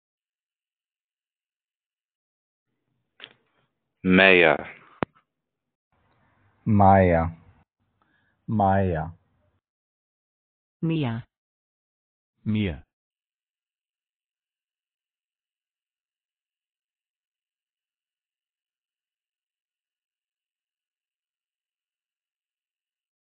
How to Pronounce Maea
This video shows you how to pronounce Maea. Record your own pronunciation, view the origin, meaning, and history of the name Maea